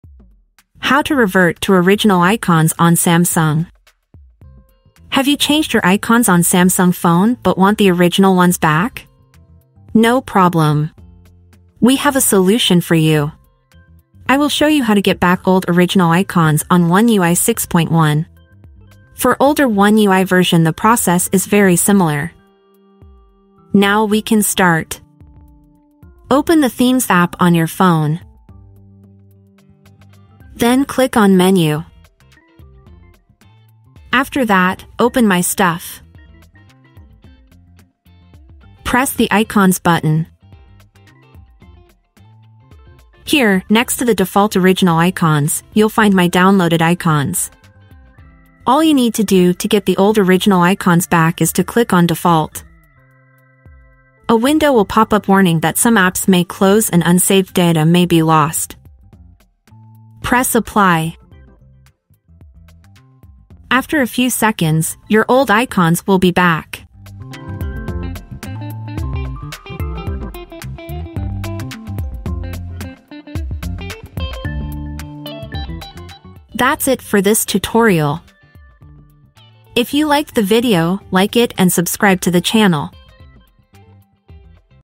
How to Revert to Original Icons on Samsung | ONE UI
Are you looking to revert to the original icons on your Samsung phone?
No problem! We've got a simple solution for you.
In this quick tutorial, I will show you how to restore the original icons on ONE UI 6.1. The process is similar for older versions of ONE UI as well.